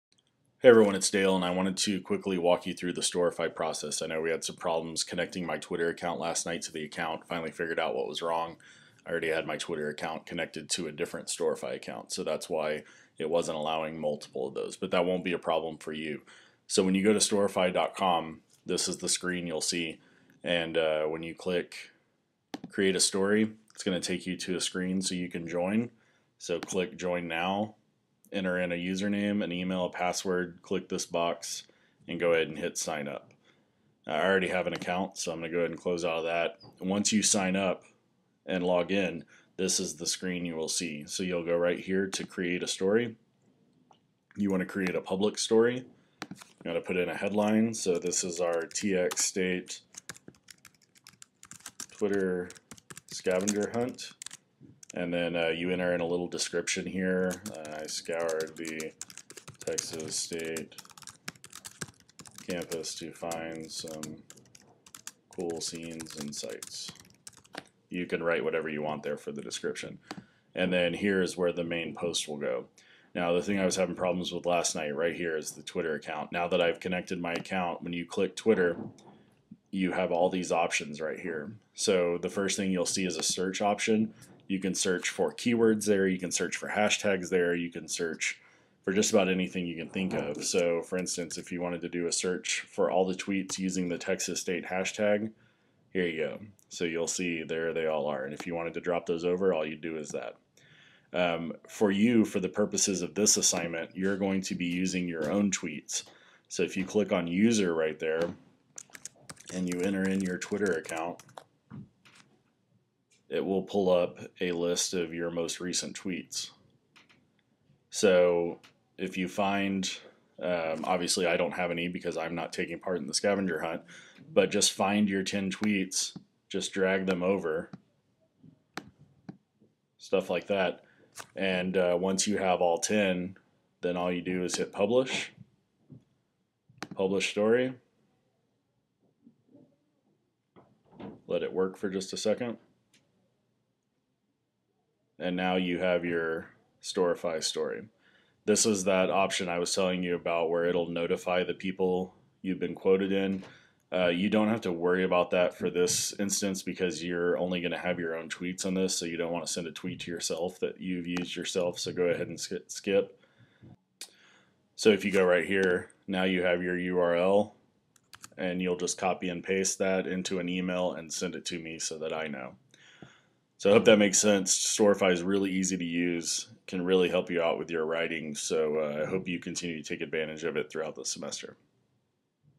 FDOM Storify Tutorial
This tutorial will walk you through the process of embedding tweets inside a Storify story.